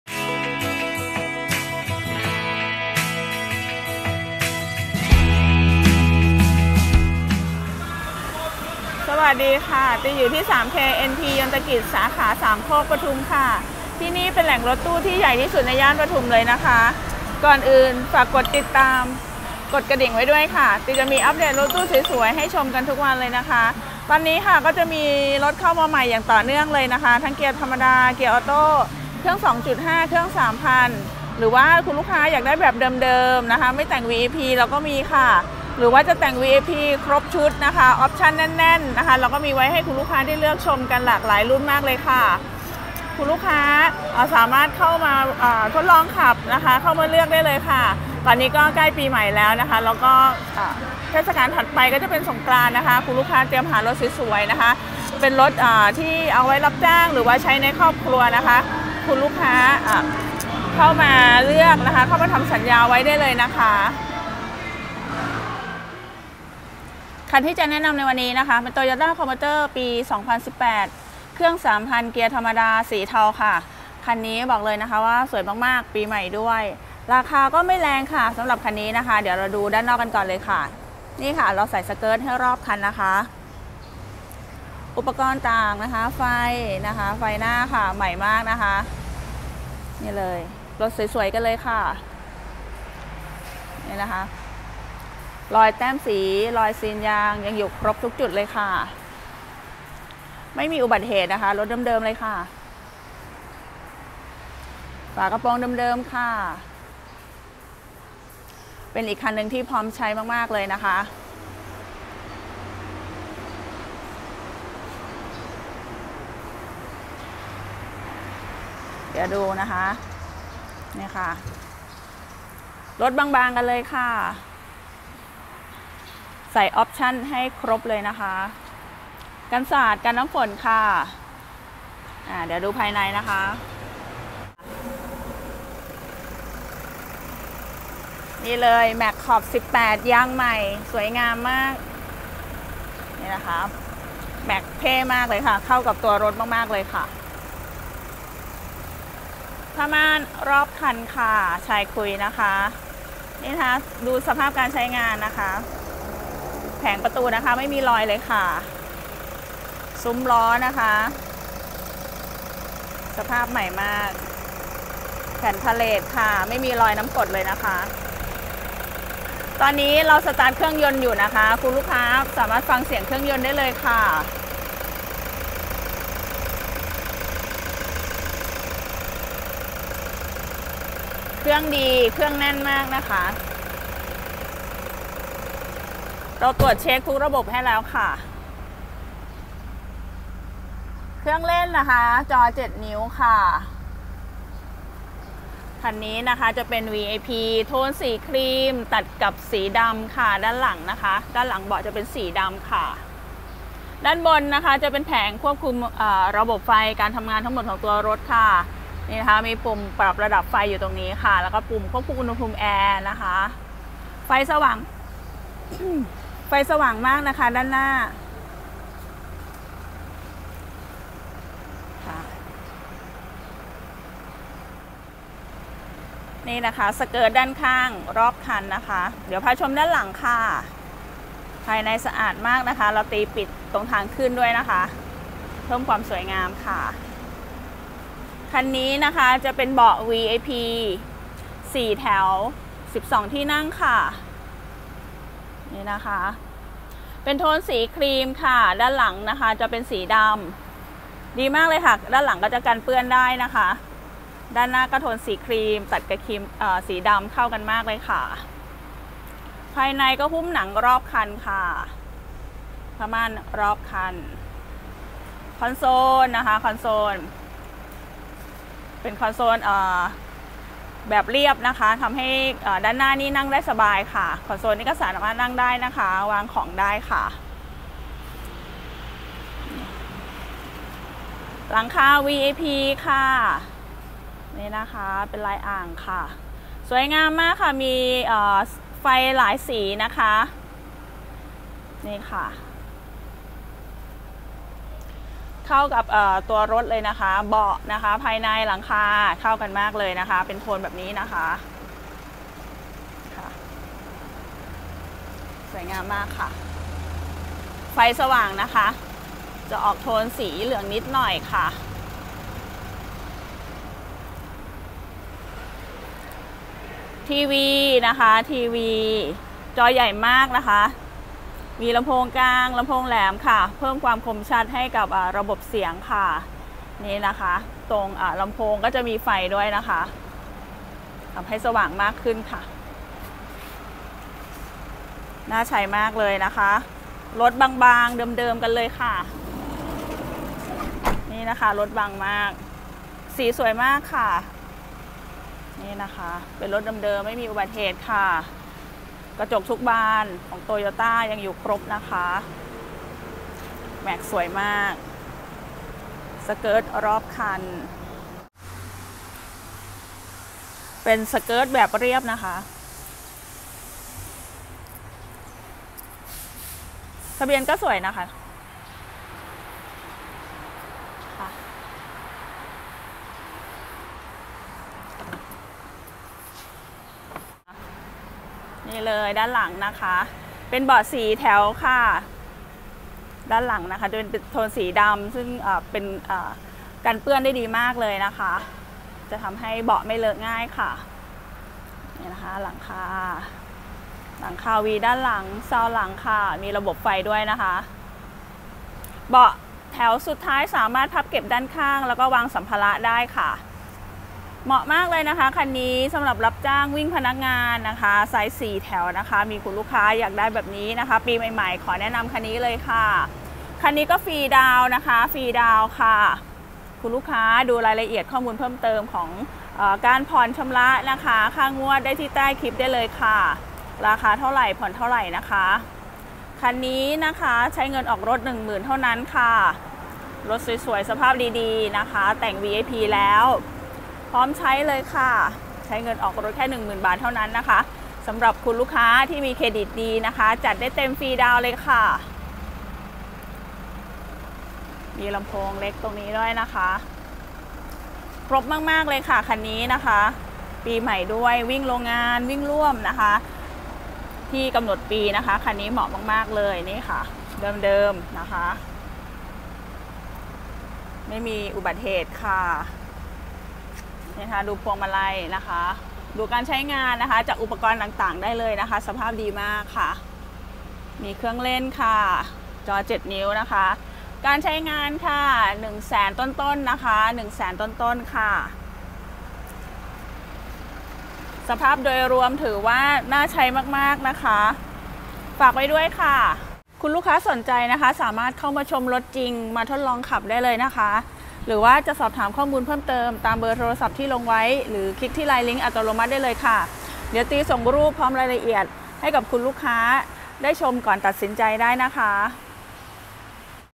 Sold out 🚫ขับสนุก ผ่อน สบาย ไม่ต้องดาวน์ Toyota Commuter 3.0Dปี2018 ดีเซล สีเทา เกียร์ธรรมดา
Toyota Commuter 3.0Dปี2018 ดีเซล สีเทา เกียร์ธรรมดา

🏅แต่งหลังคาVIP
🏅แอร์ไมโครบัส
🏅หุ้มข้างรอบคัน
🏅เบาะVIP 4 แถว
🏅ทีวี 24 นิ้ว จอพับ
🏅เครื่องเล่นจอ7”
🏅ผ้าม่านรอบคัน
🏅สเกิร์ตรอบคัน
🏅แม็กซ์18 “ยางใหม่

🌟ฟรีดาวน์
👉ค่าจัด&โอน 10,000 บาท
♦️ผ่อน 17,xxx/72 เดือน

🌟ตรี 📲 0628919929

♦️ยอดจัดอาจเปลี่ยนแปลงไปตามเงื่อนไขของธนาคารและเครดิตของลูกค้า♦️

หรือคลิ๊กที่ลิงค์อัตโนมัติด้านล่าง⤵️

ติดตามรถตู้วีไอพีเข้าใหม่ได้ที่  FB เพจเต๊นท์นะคะ🙏🙏 ⤵️